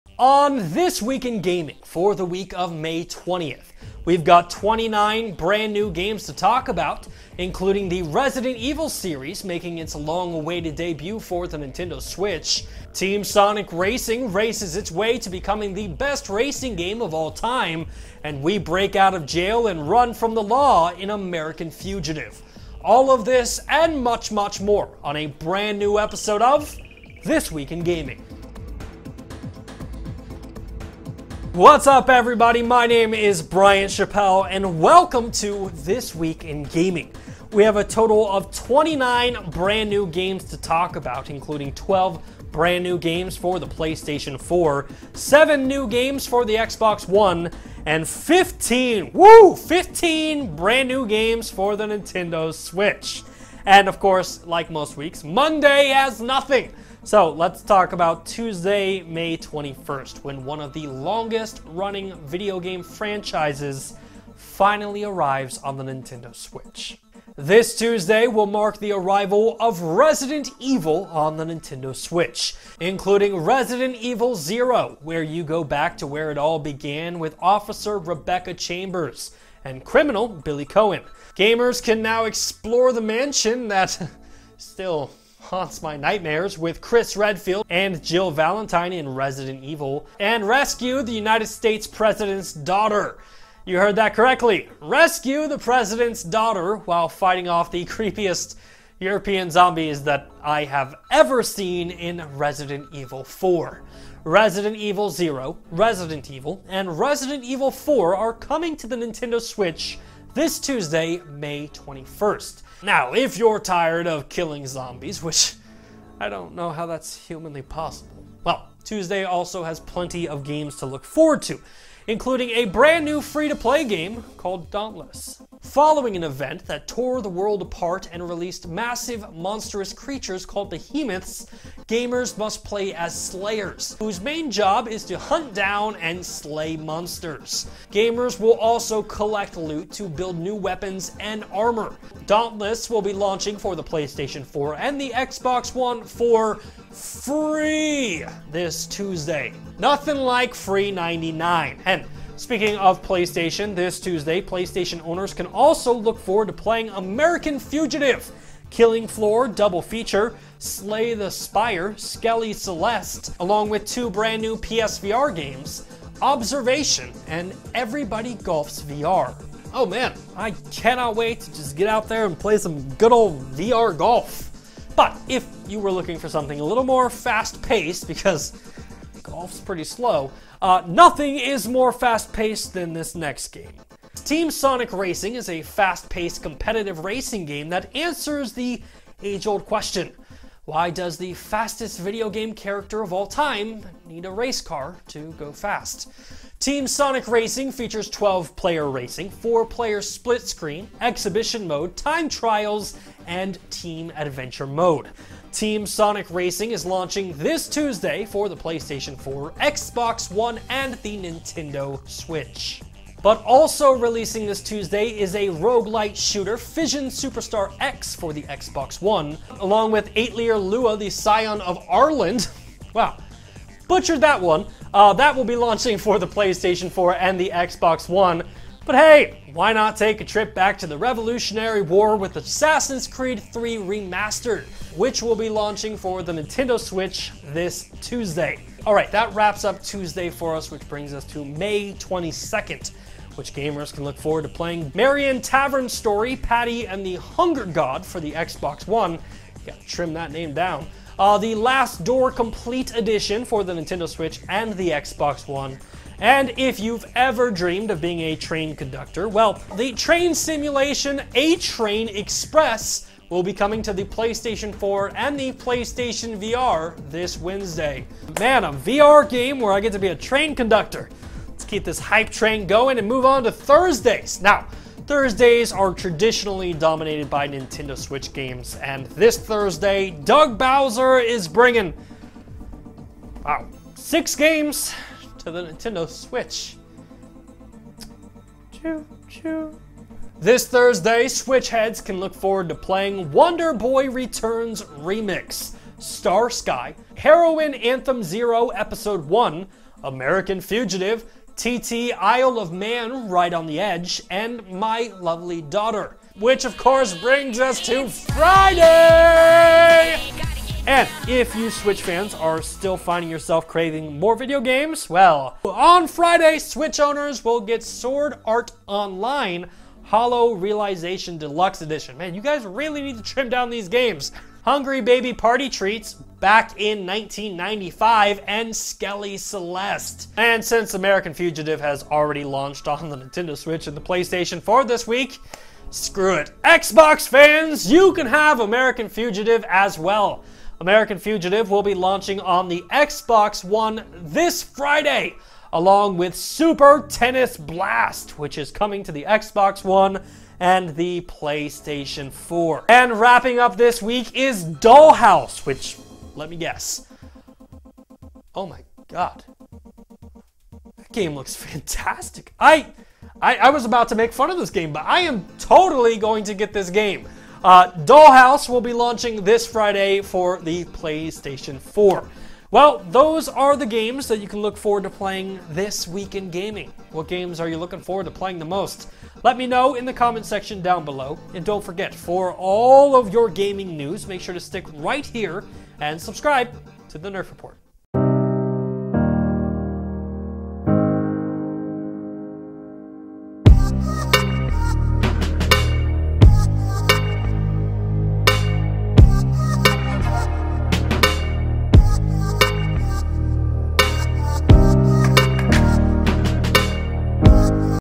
This Week in Gaming May 20th, 2019 - The Nerf Report
A complete list of every video game releasing This Week in Gaming.

This week in gaming for May 20th, 2019 has a total of 29 brand new video games releasing this week!

Get ready for a rundown of all of the video games coming out this week!

This Week in Gaming has a ton of Nintendo, PlayStation, and Xbox games on the way!

Join us as we do a quick rundown of all of the games coming to the Playstation 4, Xbox One, and the Nintendo Switch.